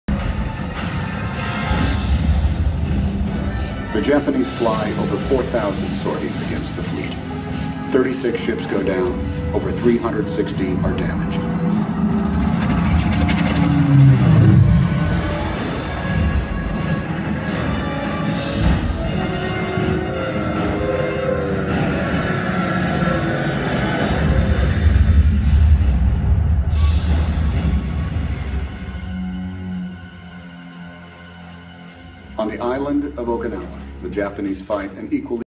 WORLD WAR II MUSEUM MOVIE
Movie shown in one of the theaters at the WW II MUSEUM in New Orleans.